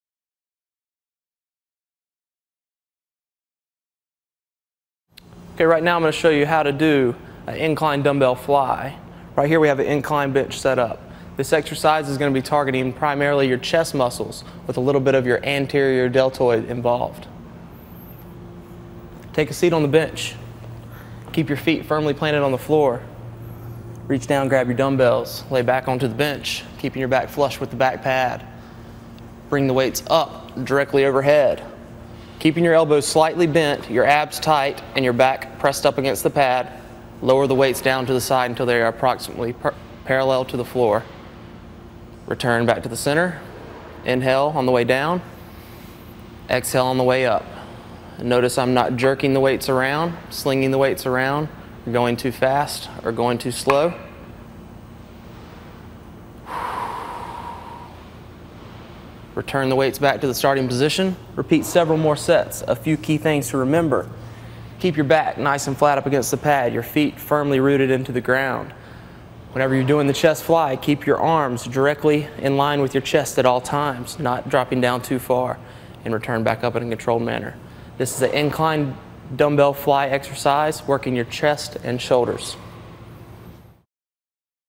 How to Do an Incline Dumbbell Fly
Try dumbbell flies on an inclined bench to work out those muscles. Learn tips and techniques for working out the chest, back, shoulders, and arms in this weightlifting video.